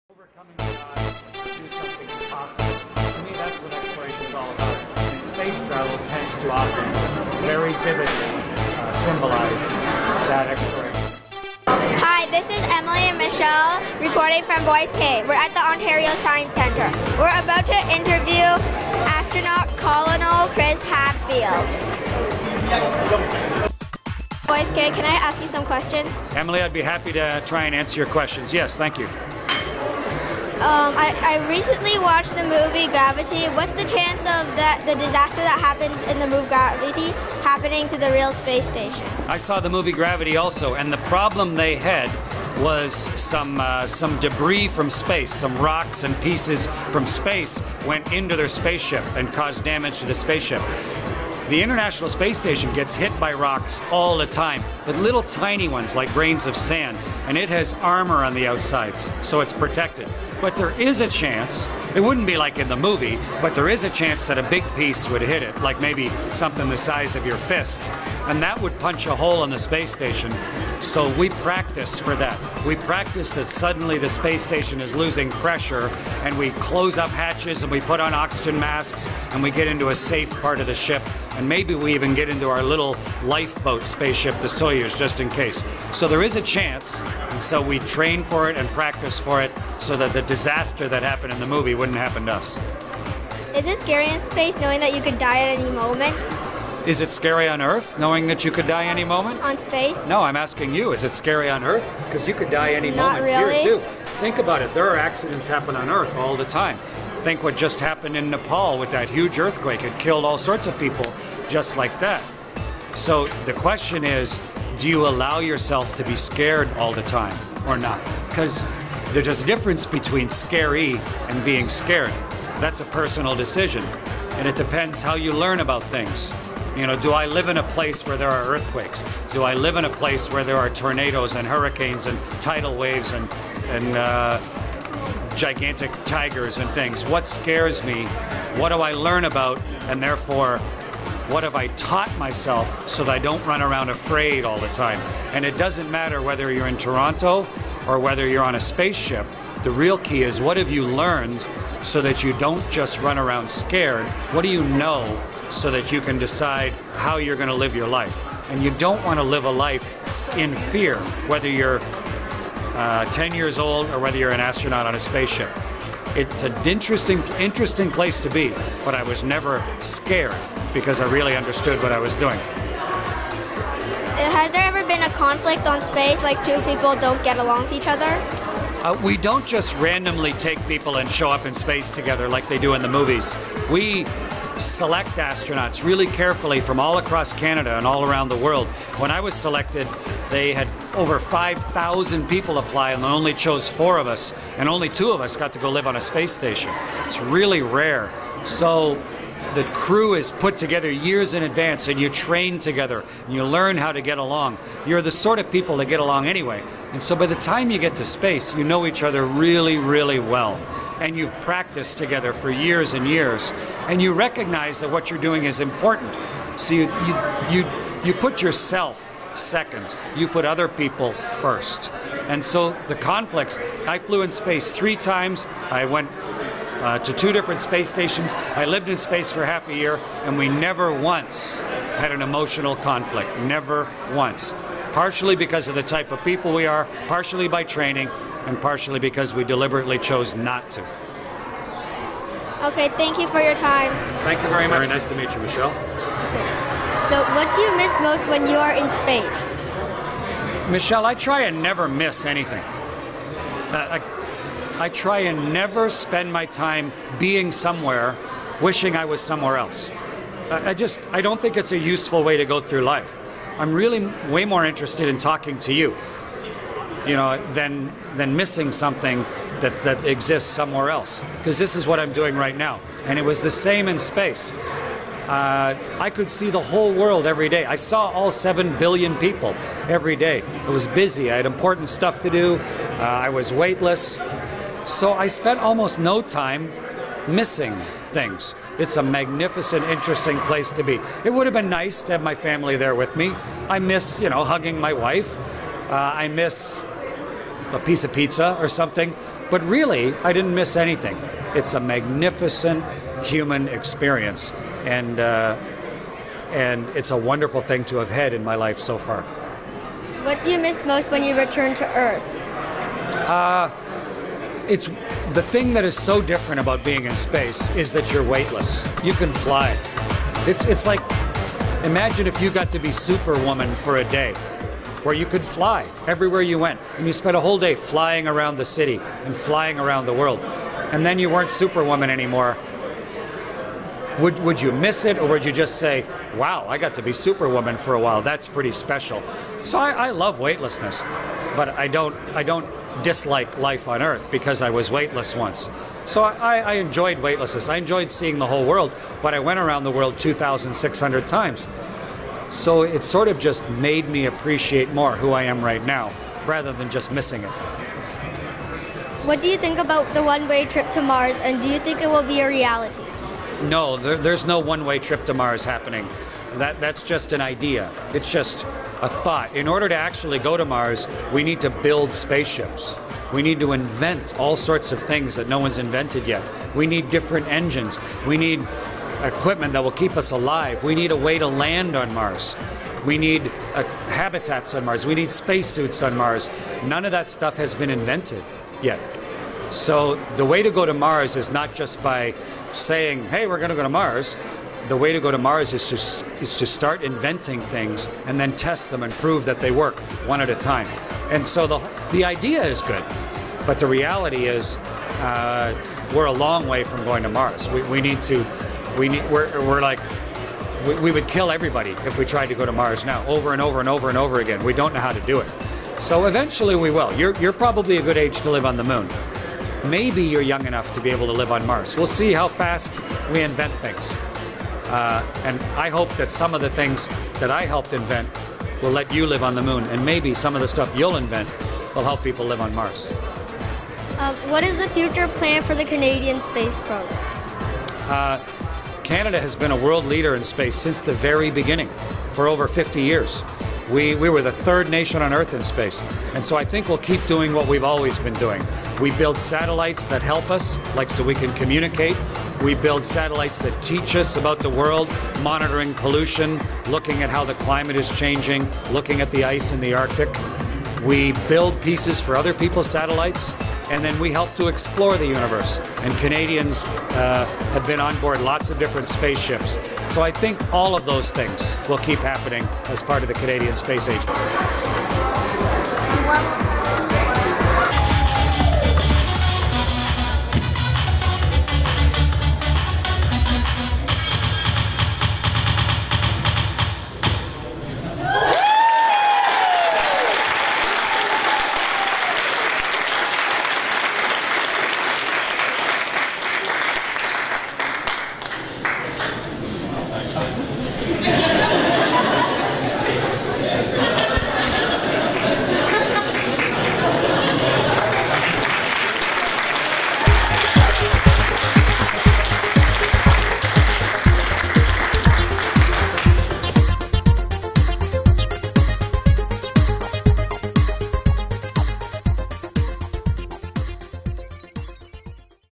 Kid Reporters Interview Chris Hadfield at Ontario Science Centre
Voice K reporters had a chance to interview Canadian astronaut, Col.Chris Hadfield at Ontario Science Centre on April 29, 2015, about his exhibition "TO BE An ASTRONAUT"